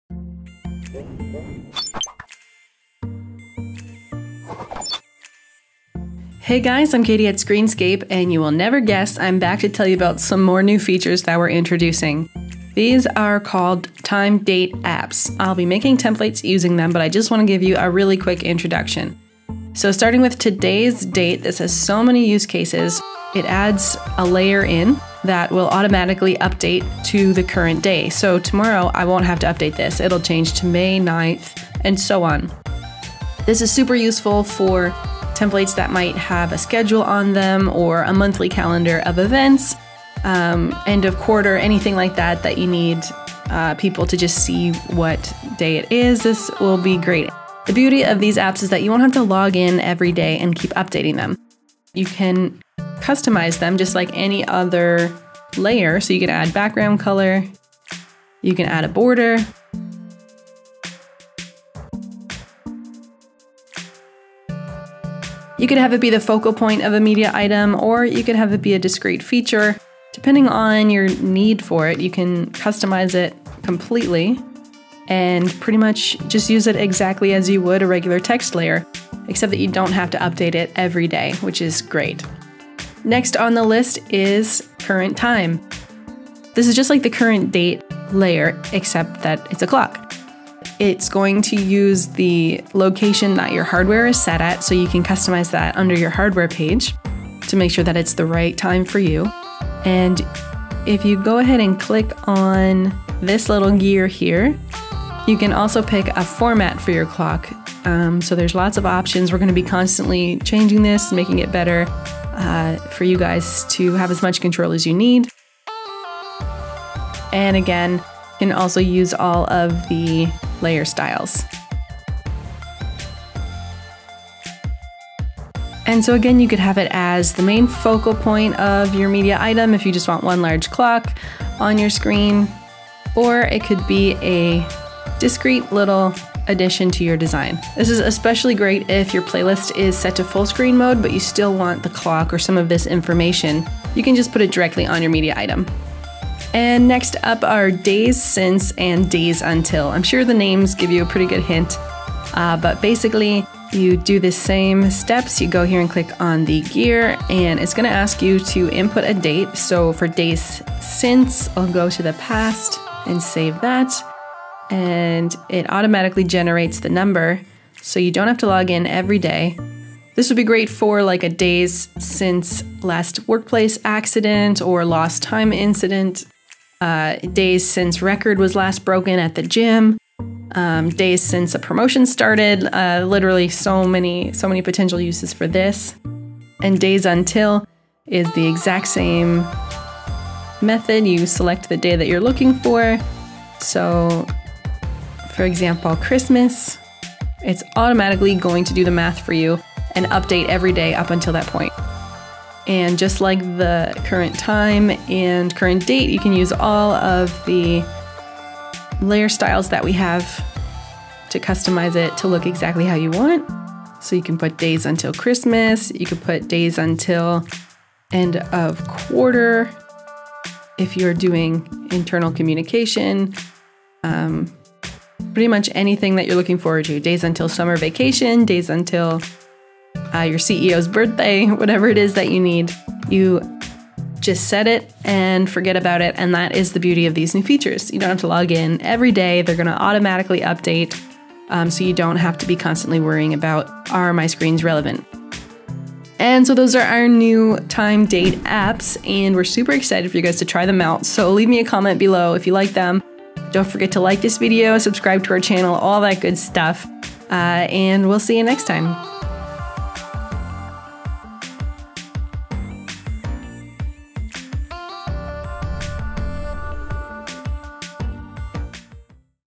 New Feature - Time and Date Templates
Guess what? We're excited to introduce ANOTHER exciting new feature - Time/Date apps in our templates!

Now you can add a clock, the date, days since or days until directly on your design - This is especially useful if you play your media in full screen mode!

There are a ton of use cases for these templates. You could put the time or date on a schedule, post days since last workplace injury, or days until company picnic - anything!